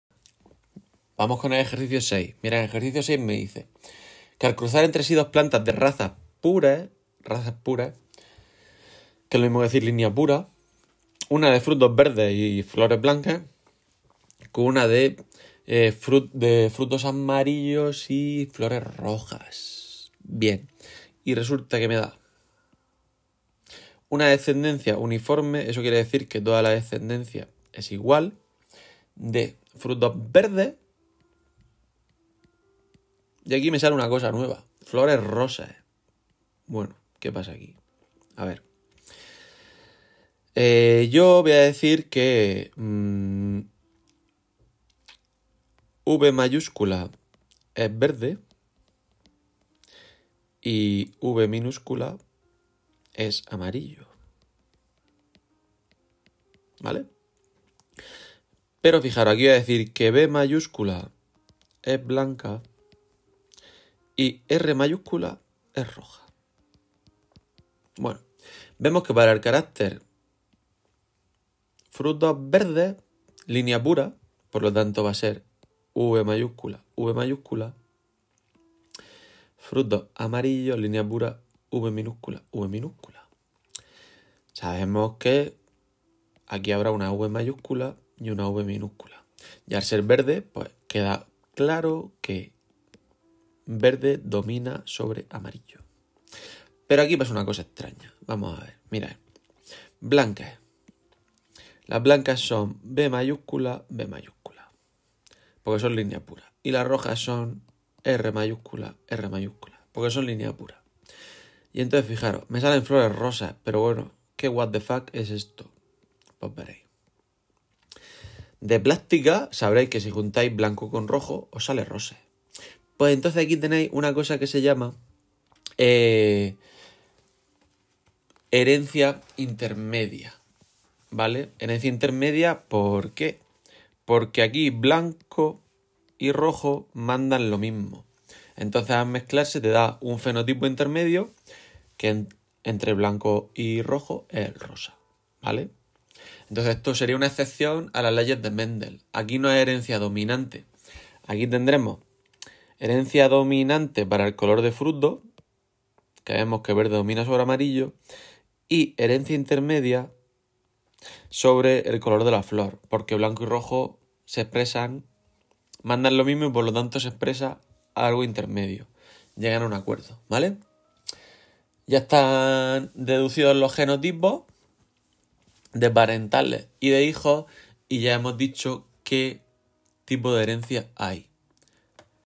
Ejercicio 6 Boletín 1 Genética
Al cruzar entre sí dos plantas de razas puras, una de frutos verdes y flores blancas con otra de frutos amarillos y flores rojas se obtiene una descendencia uniforme de frutos verdes y flores rosas
¿Qué tipo de herencia se está observando?
Deduce los genotipos de parentales e hijos